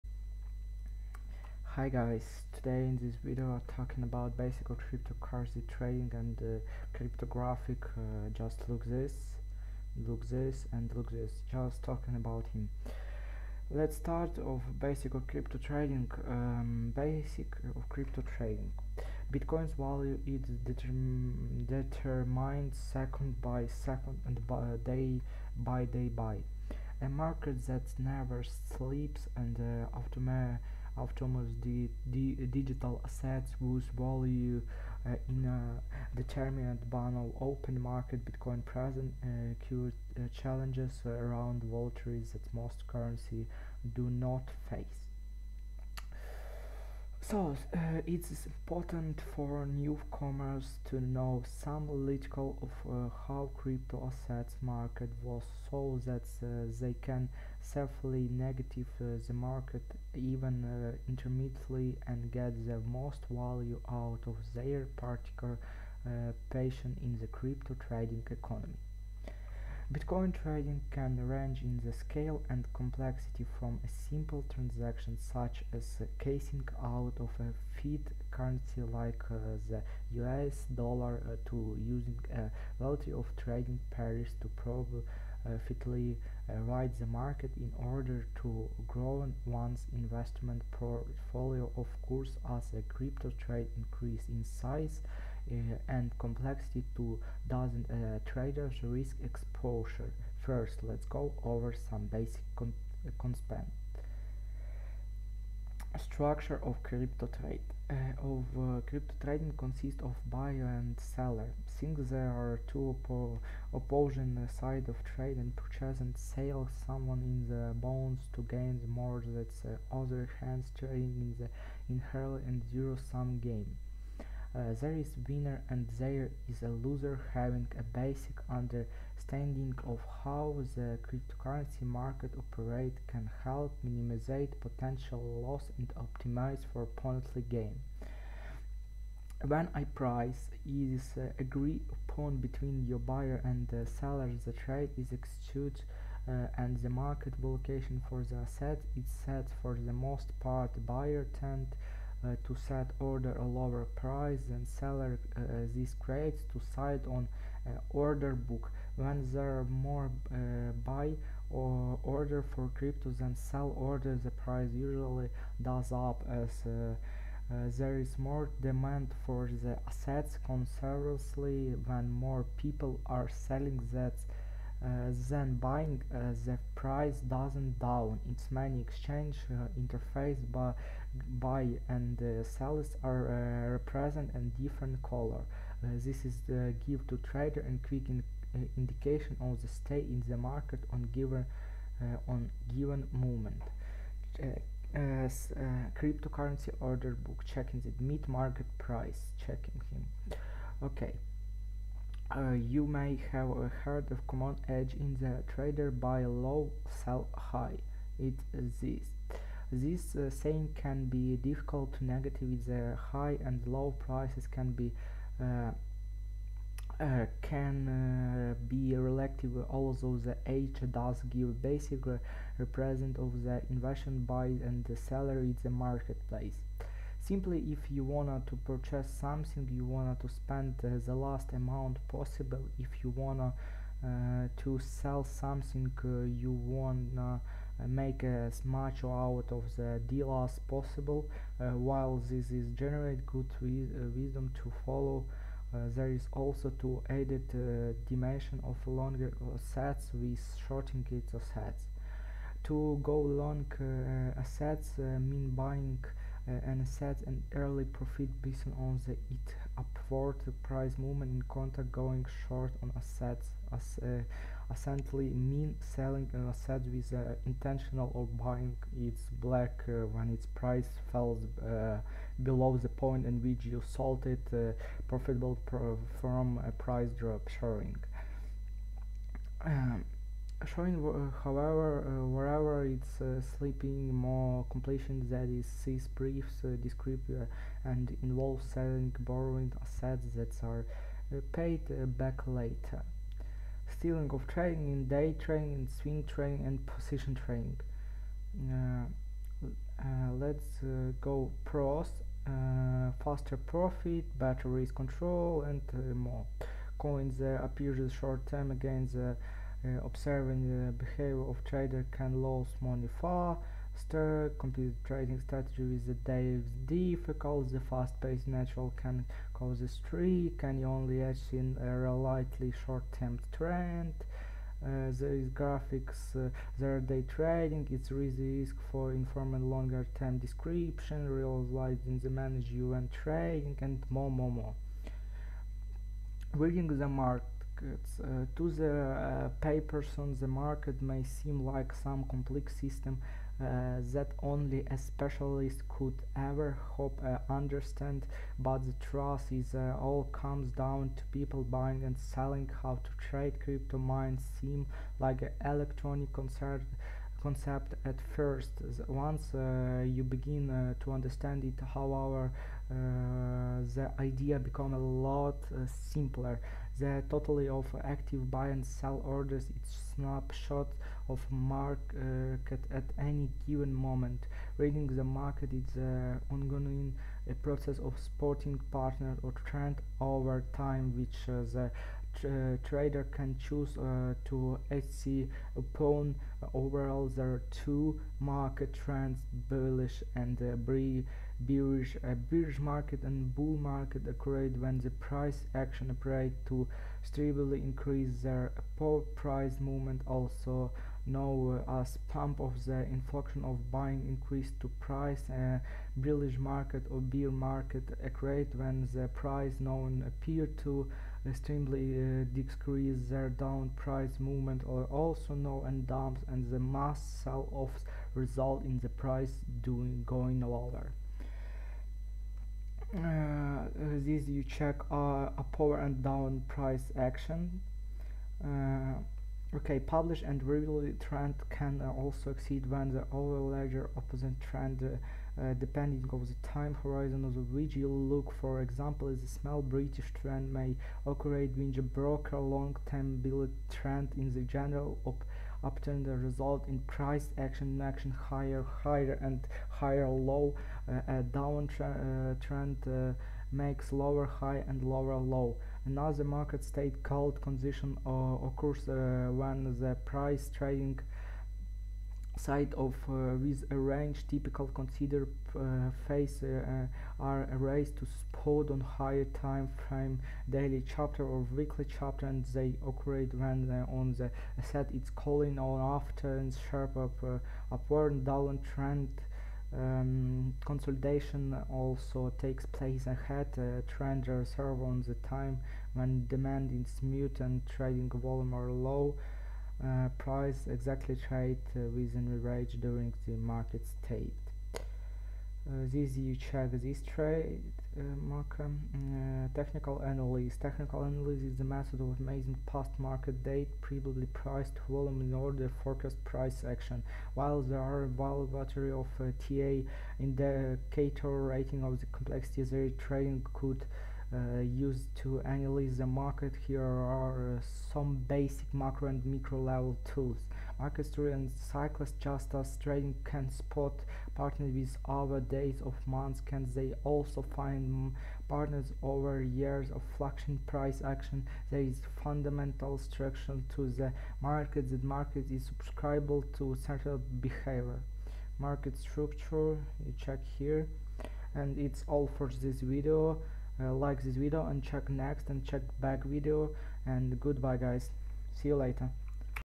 About basic trading
In this video, I talked about the basics of trading and what you will have to face